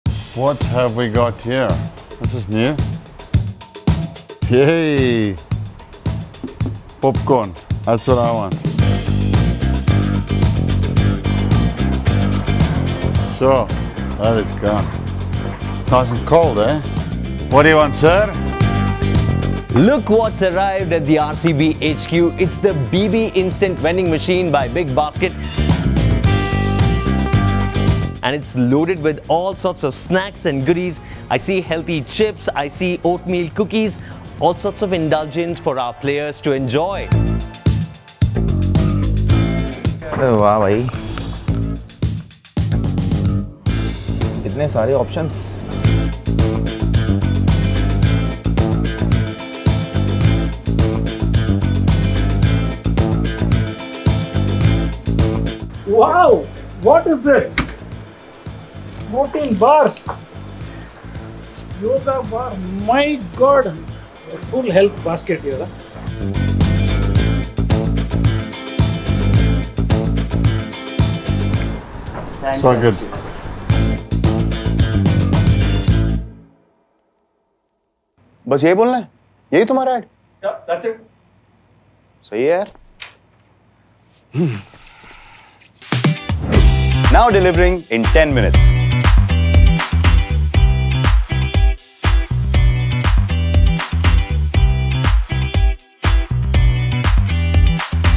BB Vending Machine at RCB HQ | IPL 2024
The BB Instant vending machine from @bigbasketofficial was an instant hit with the cricketers at RCB HQ. 🧃🧋🍟

Download the Big Basket App and get gourmet food products, groceries and electronics delivered instantly! 📱